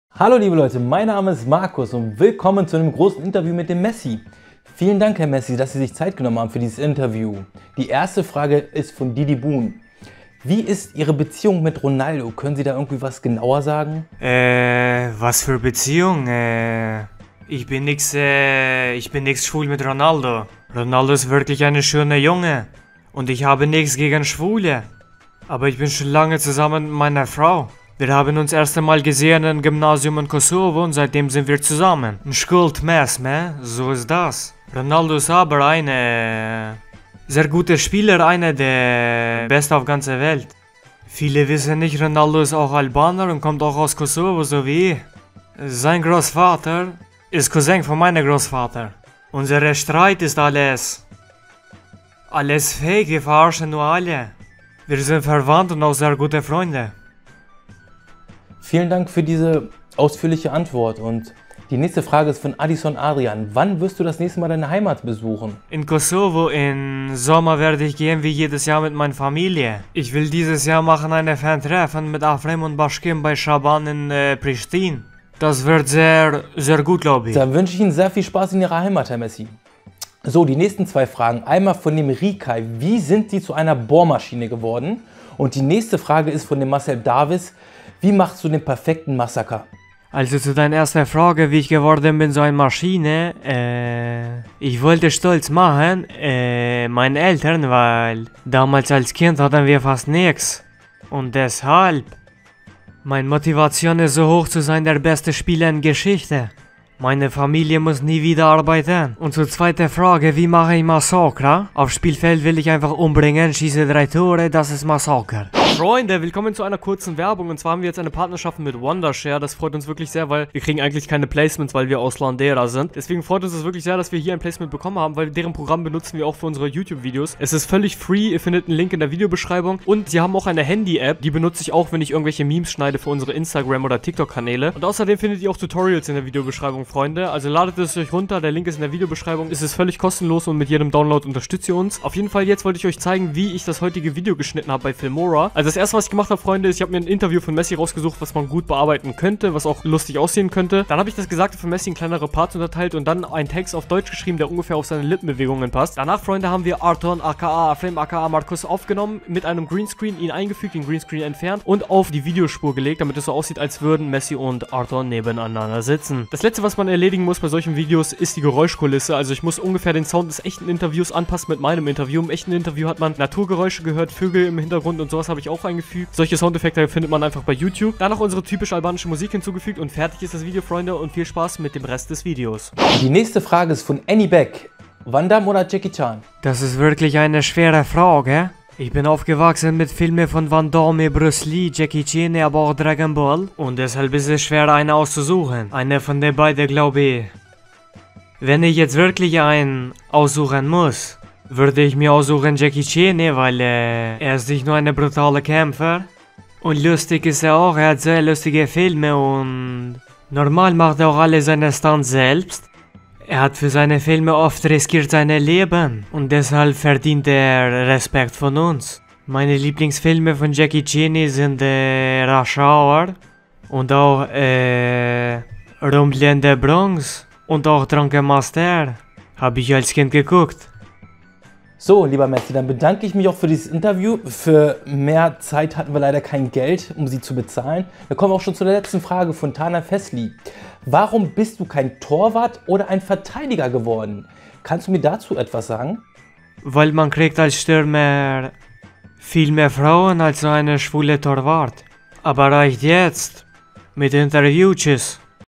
Messi im Interview mit Markus! 😂🇦🇱 Kommt er wirklich aus Kosovo? 😂
Creator Code: bardh_g

► Produzent:
Team KüsengsTV